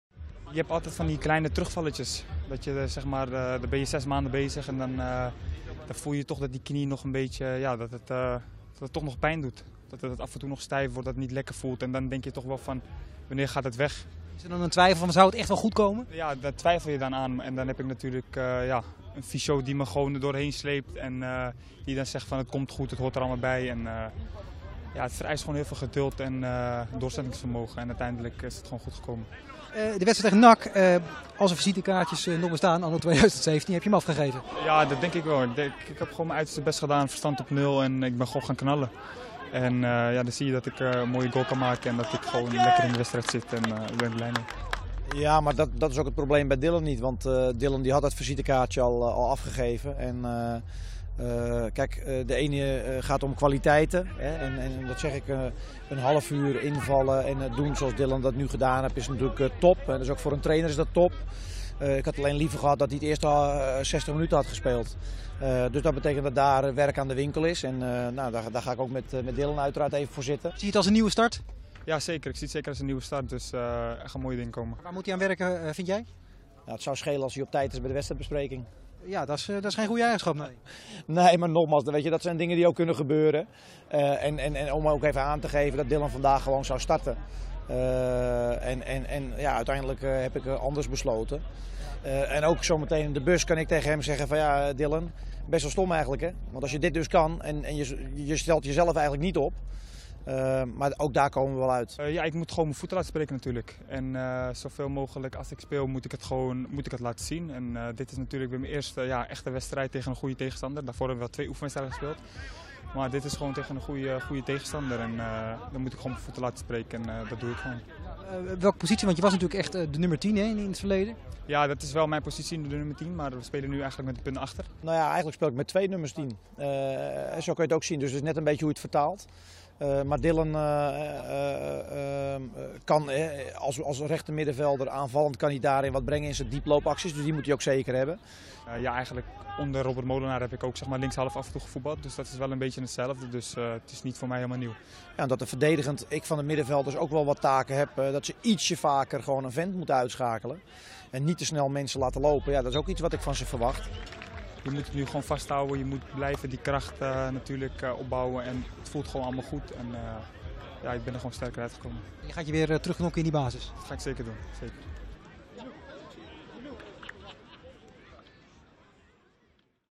Nieuwe start voor Volendam-speler Dylan Mertens
Nieuwe start voor Volendam-speler Dylan Mertens na zware kruisbandblessure